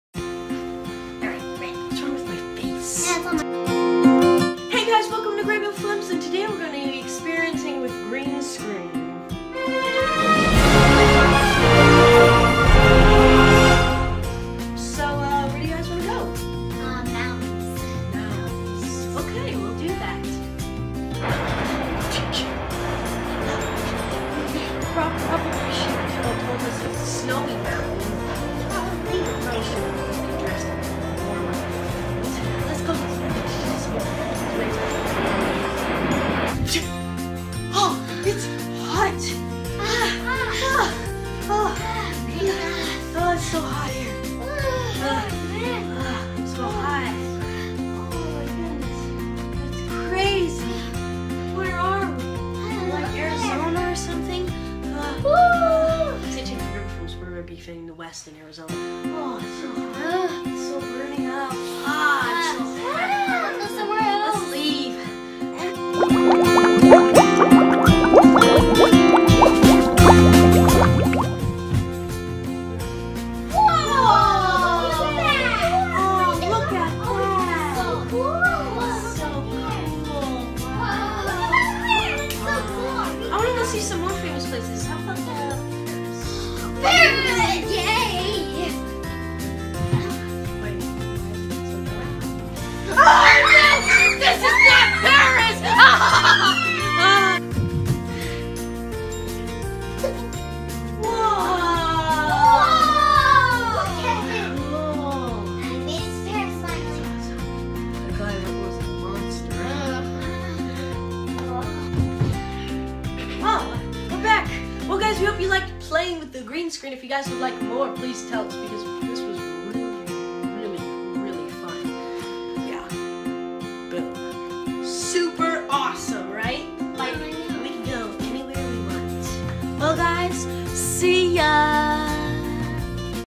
Green Screen Travel
I got a green screen and decided to check it out.